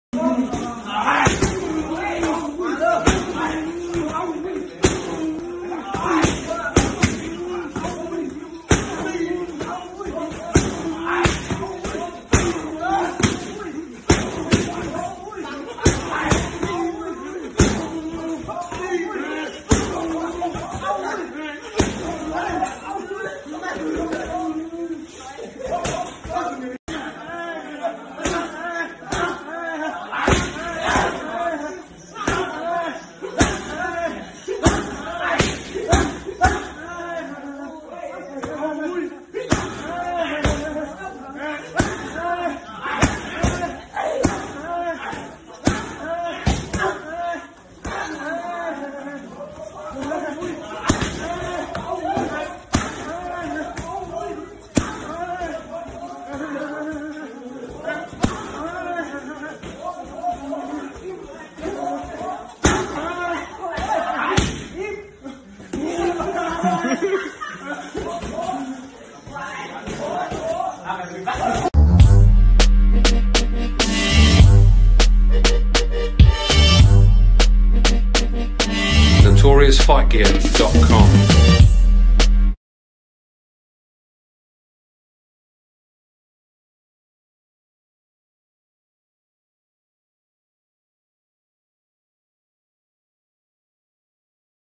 Superlek hitting pads its just too cool. Awesome Muay Thai fighter.
You can't doubt for a second that in Thailand they kive and breath Muay Thai.

So for one fighter to reach the level that Superlek has just shows the level of his dedication.

Awesome fighter.